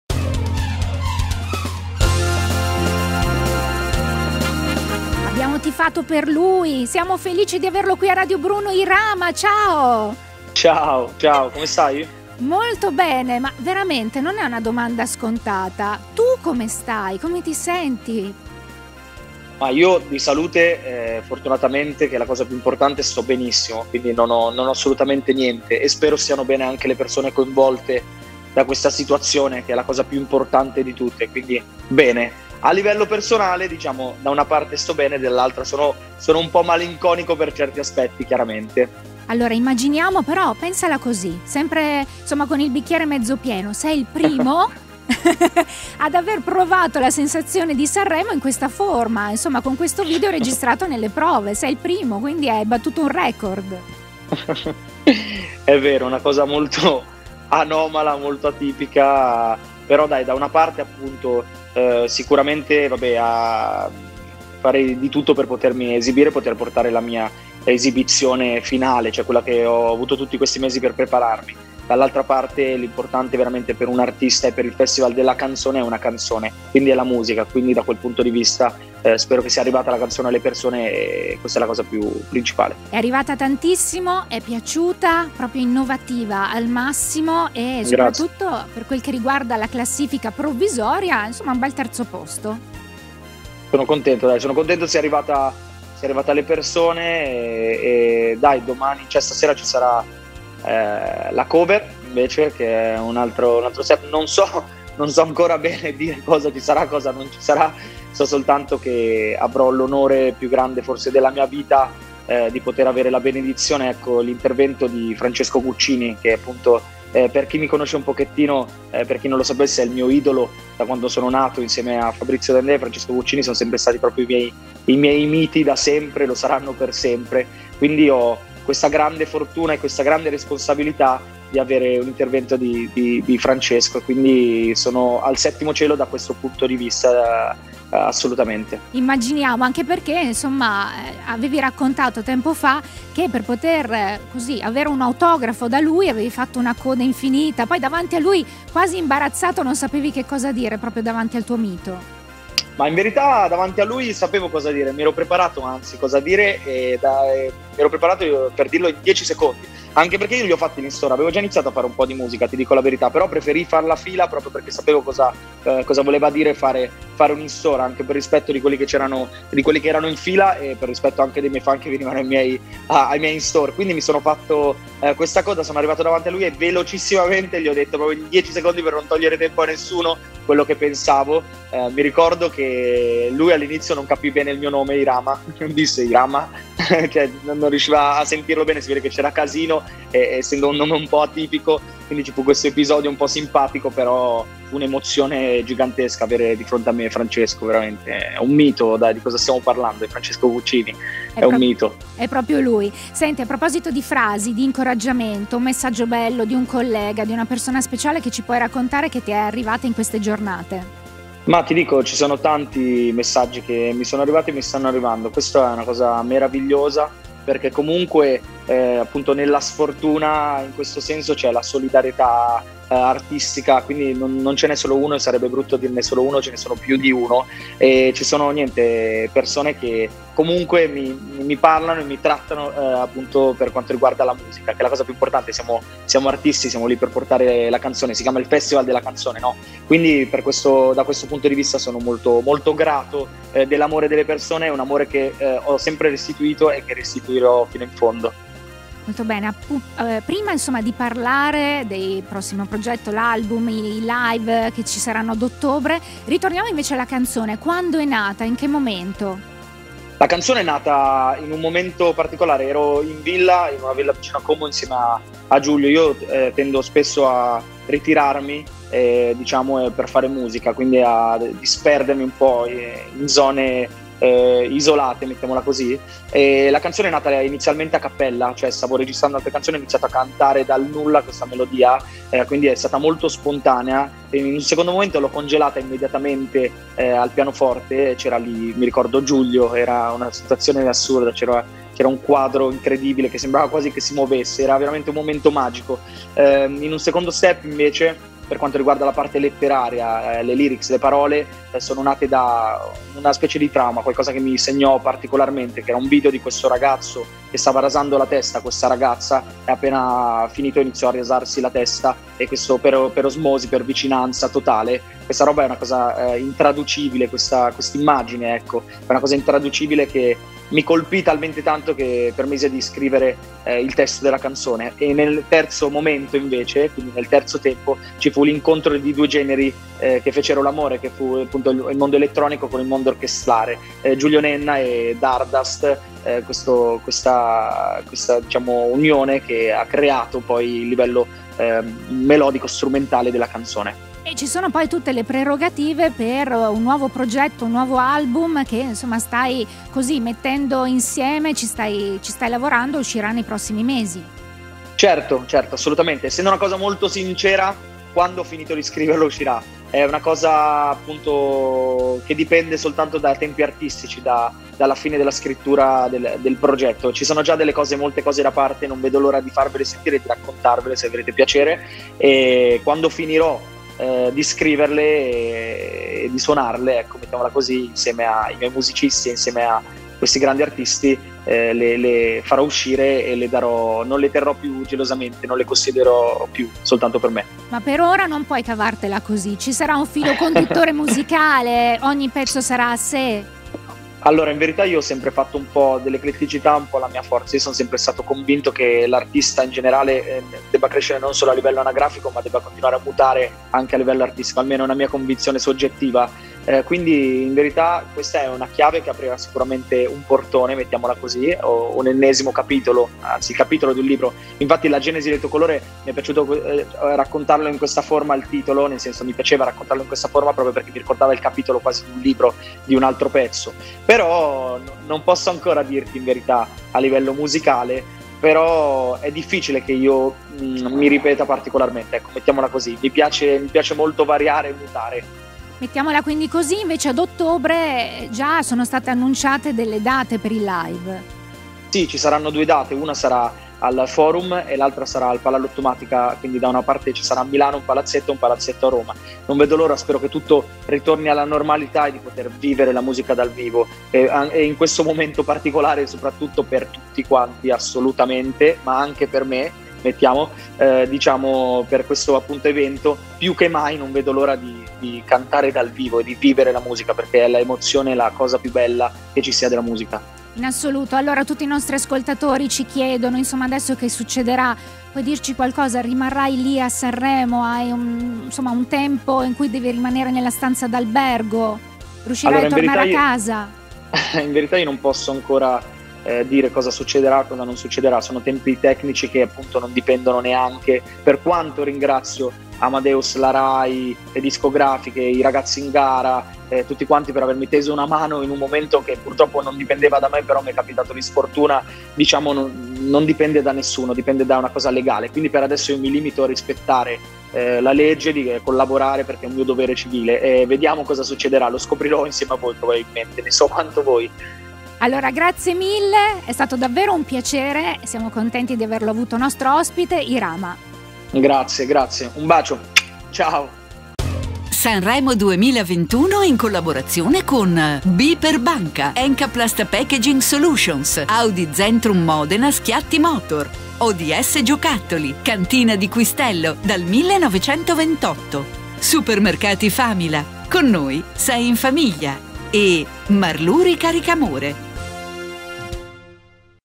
Sanremo 2021 - Irama
Guarda l'intervista di Radio bruno a Irama